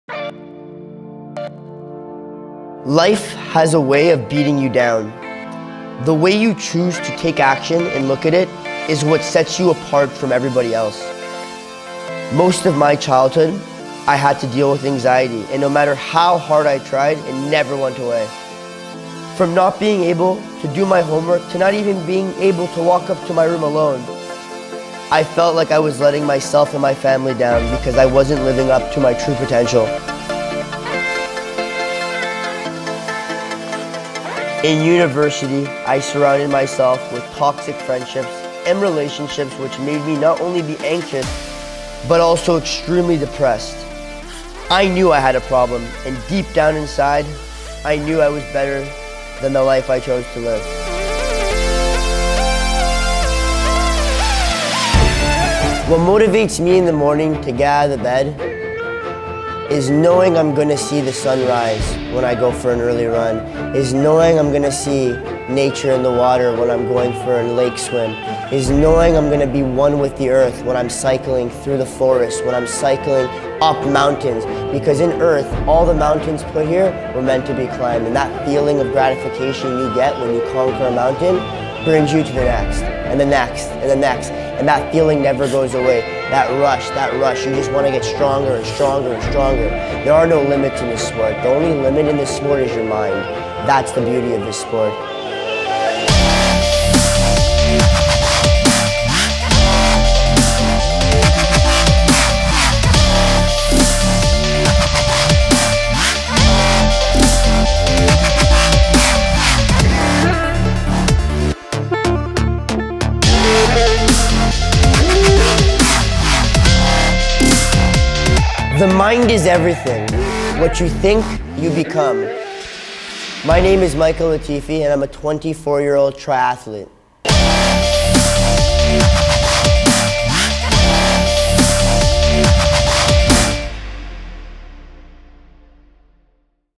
Meet Michael Alexander Latifi
Follow Michael on this inspiration journey proving that the mind is everything what you think you become.Video by Pedja Milosavljevic by Jeremie Dupont